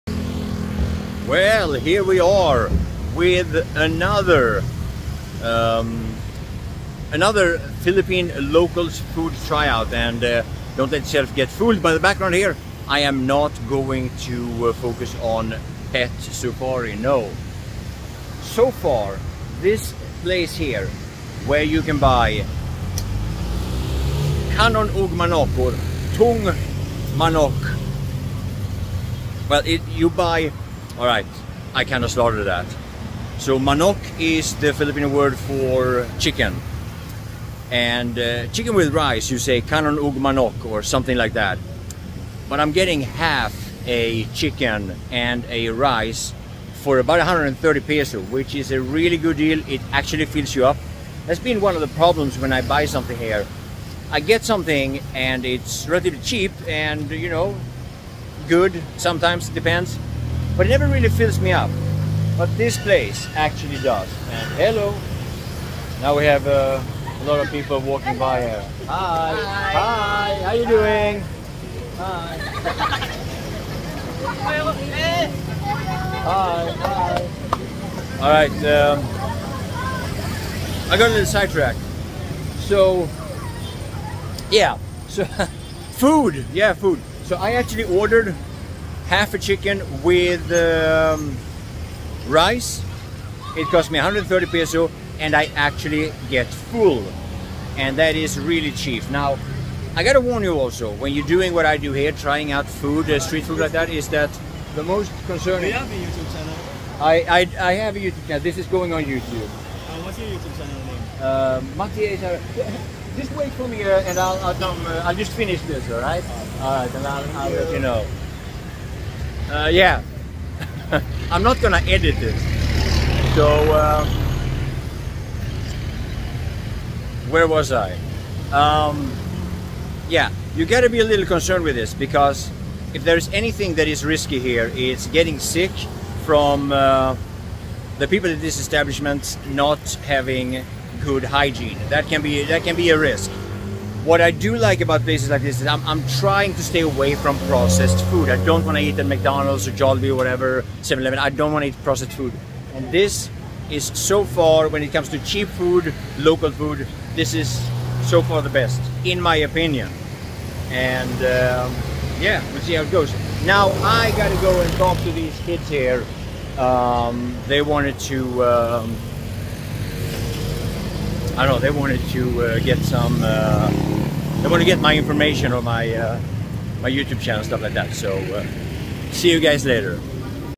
Philippines - Local food
My main channels for gaming

This place is close to Robisons grocery store in the direction of SN Mall Lanang
In bisaya Half a chicken with rice is: Tunga sa manok ug kan'on

Always be careful and observe the hygiene or lack there off when buying
food in tropical countries like this